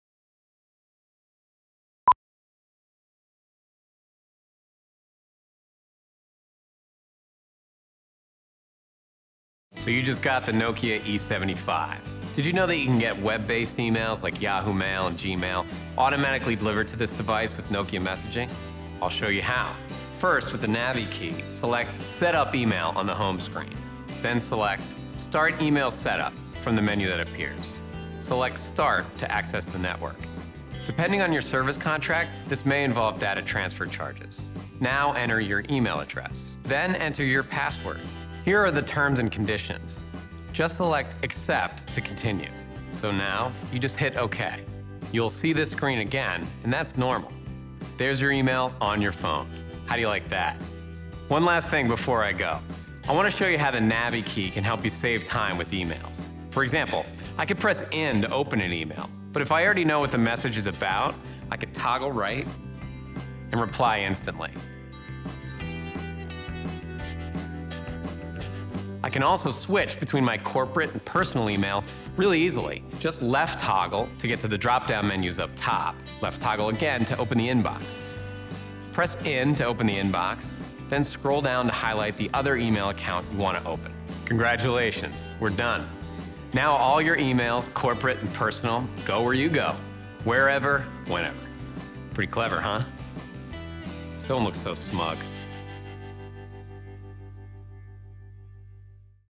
Nokia E75 - Tutorial zur Einrichtung von mobilen E-Mail-Diensten wie Nokia Messaging
Dieses Video zeigt Ihnen einfach und anschaulich wie Sie auf Ihrem Nokia E75 die Nutzung von mobilen E-Mail-Diensten wie Nokia Messaging einrichten.